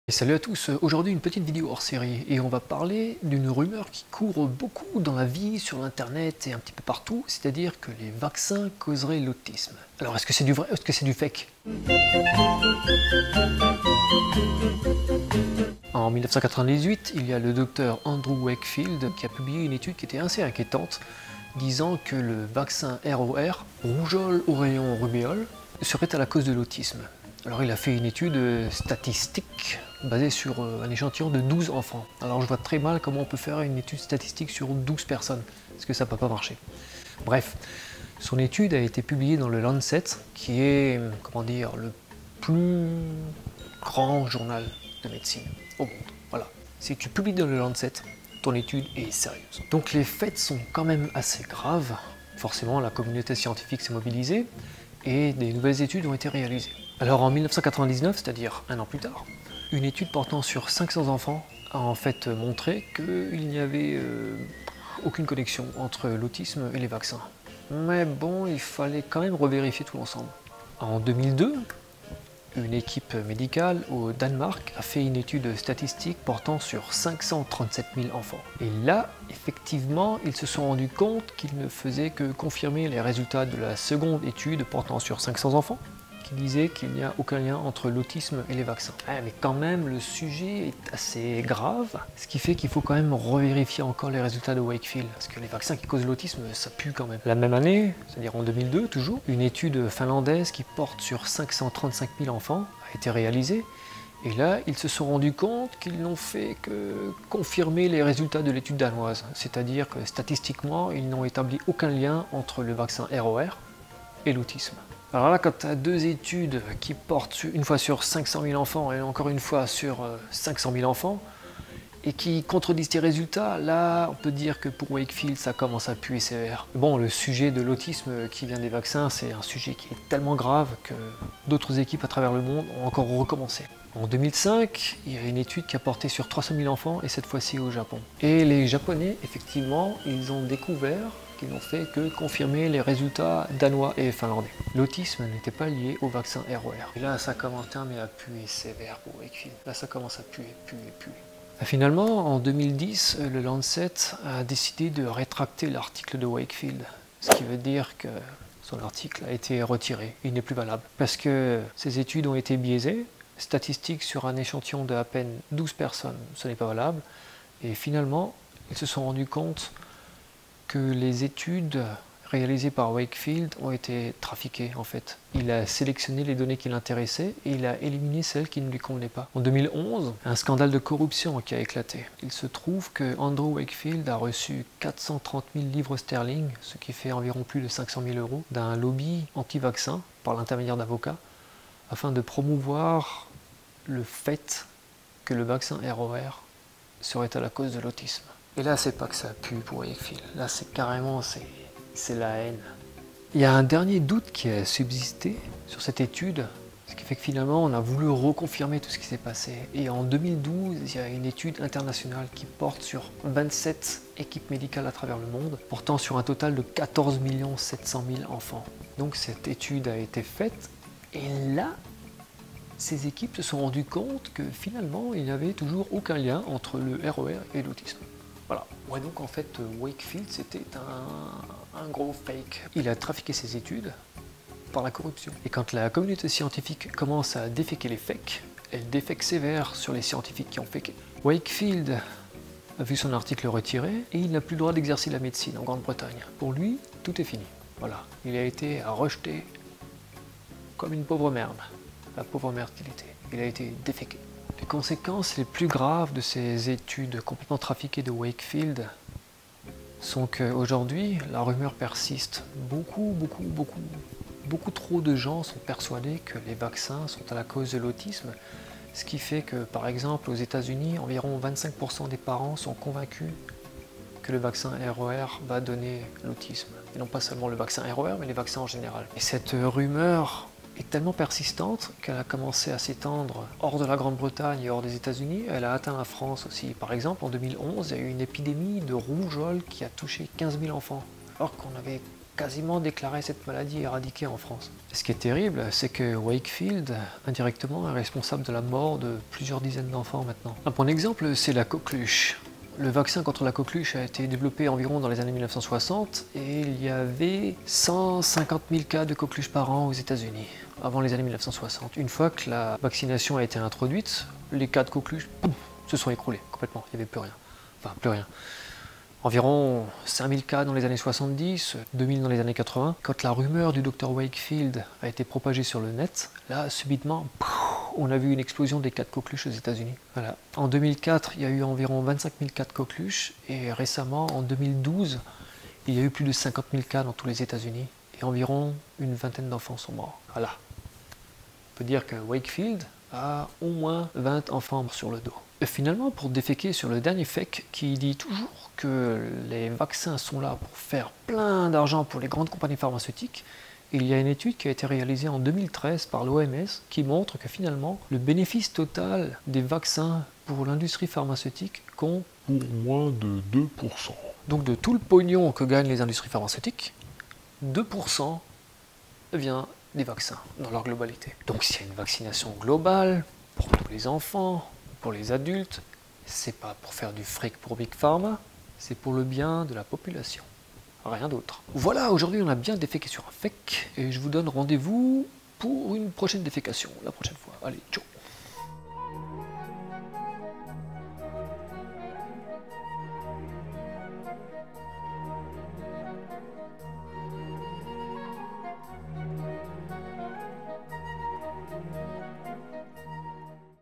08 * Les vaccins favorisent-ils l'autisme?
Les vaccins favorisent l'autisme! Ah... une légende urbaine, mais dramatique... Elle mérite d'être "défakée" :)

Liens vers les articles cités dans la vidéo (tous peer-reviewed):
Autres articles:
* Affaire de corruption de Wakefield:
Le premier qui conteste l'existence d'une maladie avec moi, je lui fait ça

Comment les vaccins causent-ils l'autisme?

Cette vidéo est inspirée du travail de Defakator, le super-héros qui defake sur les fakes! Allez regarder sa chaîne, ça défake sévère!